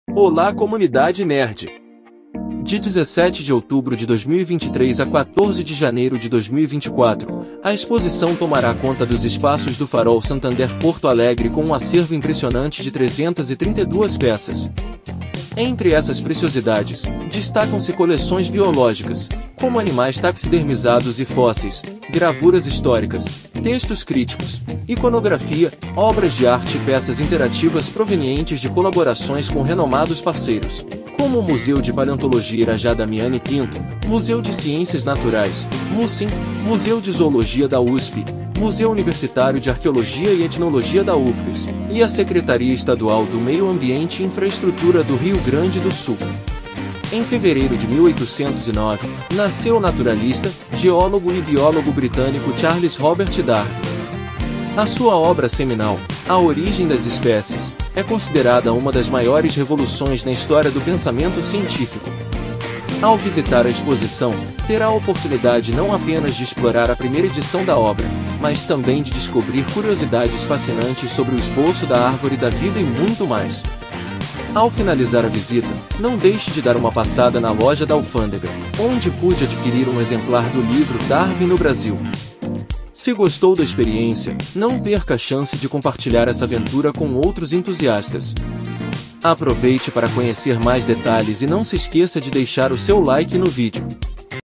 Darwin Origens & Evolução
Visitação: de 17 de outubro de 2023 a 14 de janeiro de 2024
Farol Santander – Rua Sete de Setembro, 1028 – Centro Histórico, Porto Alegre